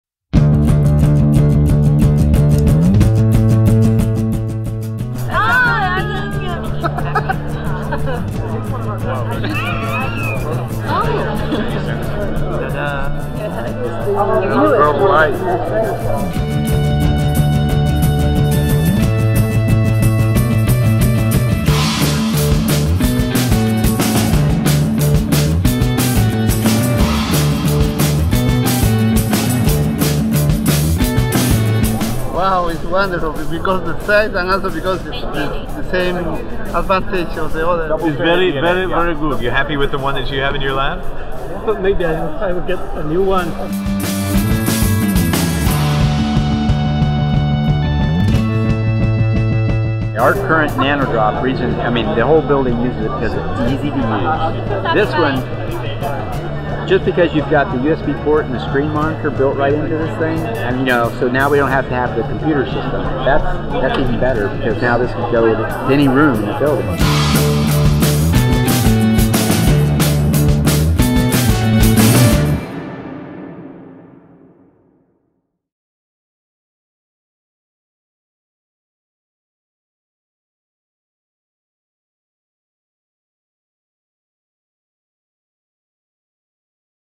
NanoDrop Lite First Impressions
Here are scientists' first reactions to the new Thermo Scientific NanoDrop Lite at its unveiling at the 2012 Plant and Animal Genome conference.  for more information on the NanoDrop Lite!